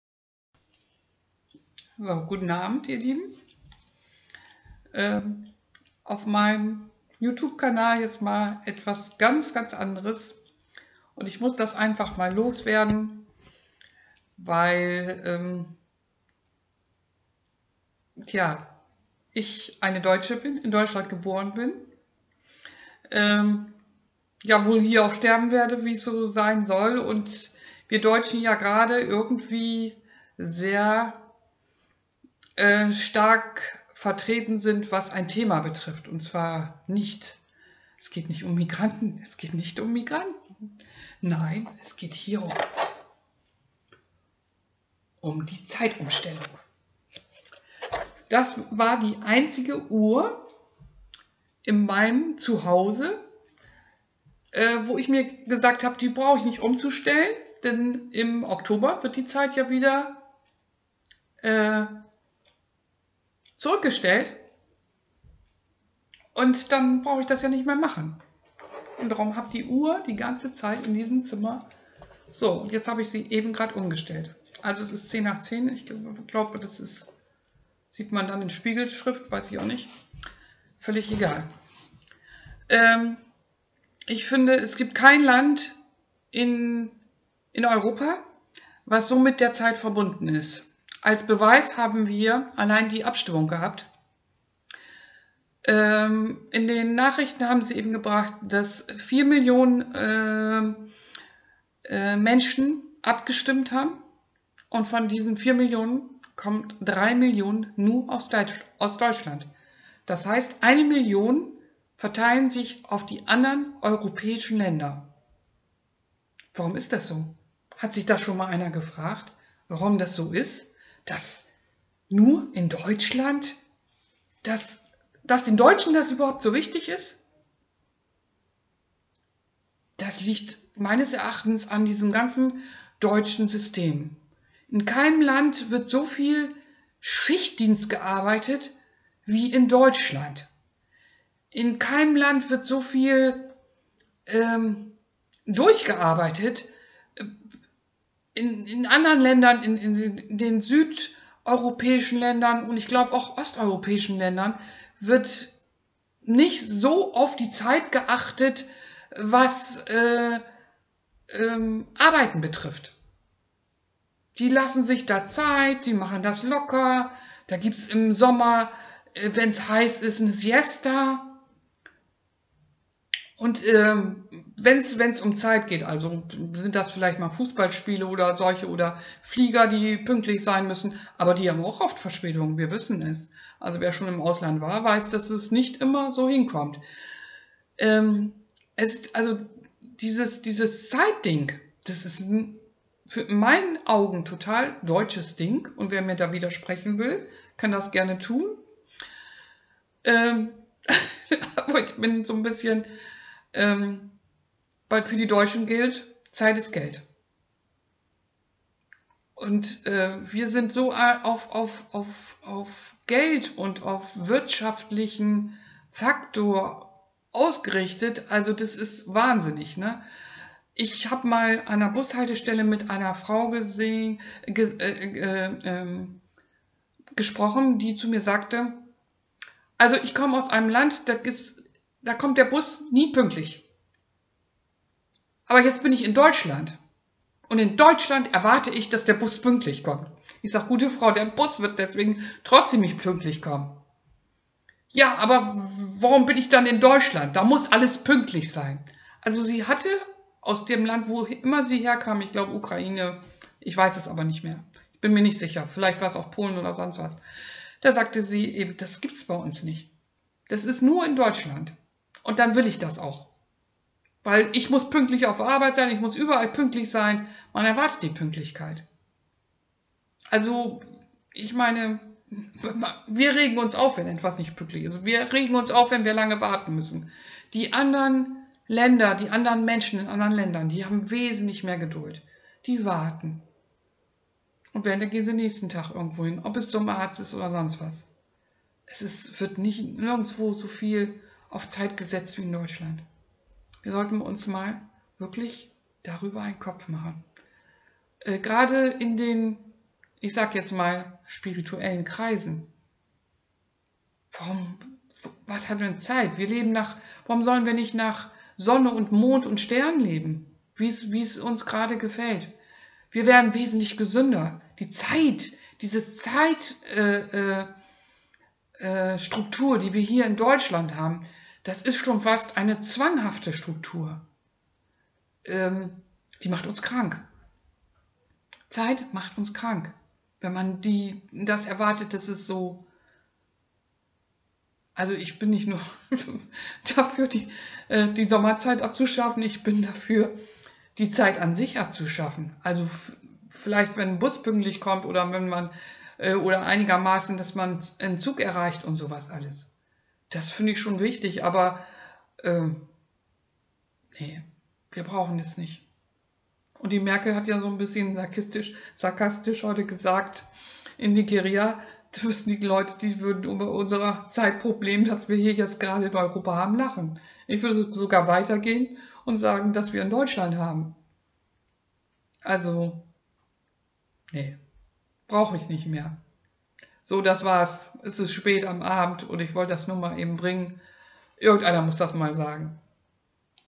Zeitumstellung
Aufhebung der Zeitumstellung in Deutschland
Warum gerade in Deutschland die Zeit so wichtig ist.